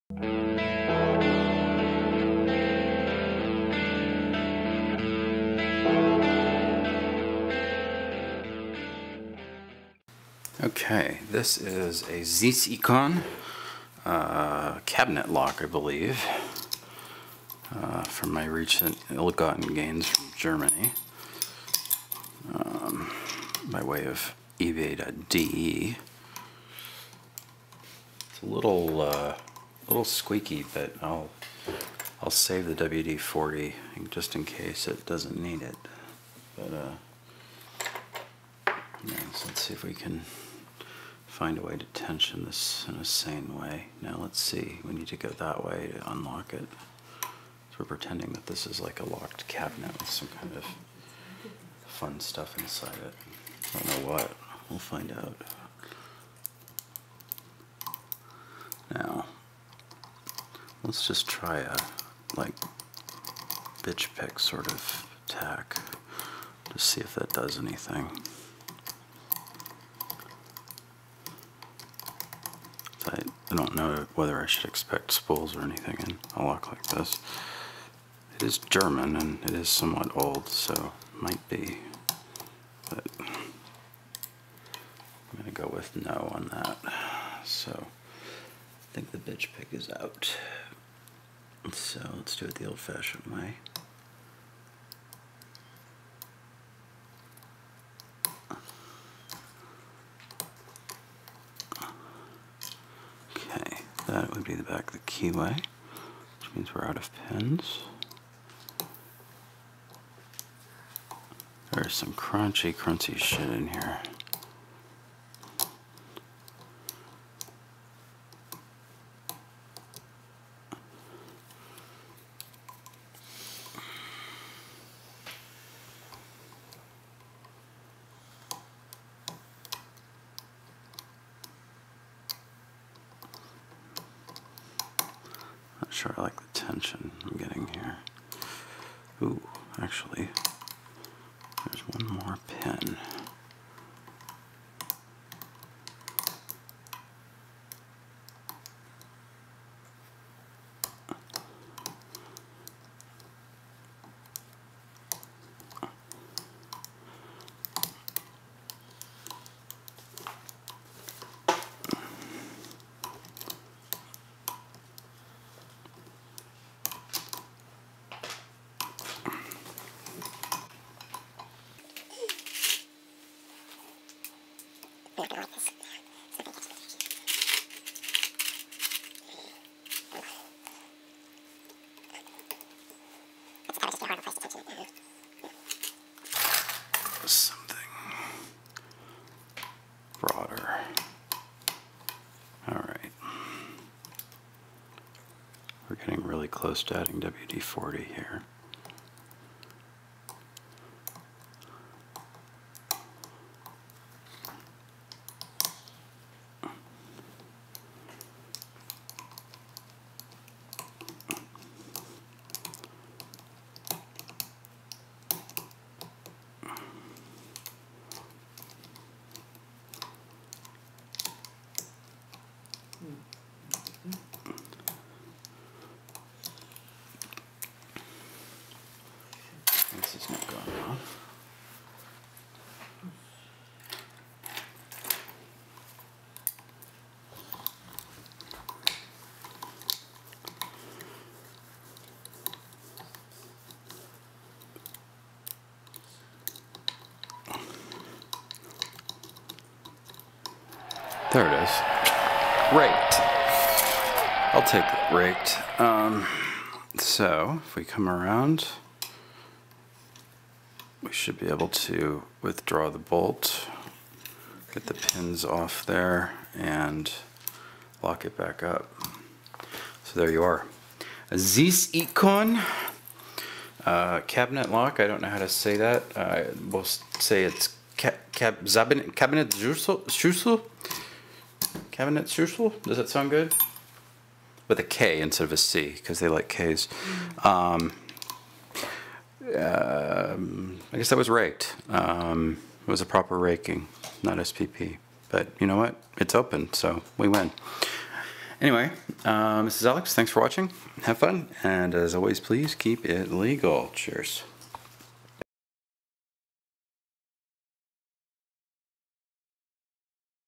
(188) Zeiss-Ikon (ZI) Cabinet Lock SPP/MPP
A cool old German cabinet lock made by the venerable Zeiss-Ikon company.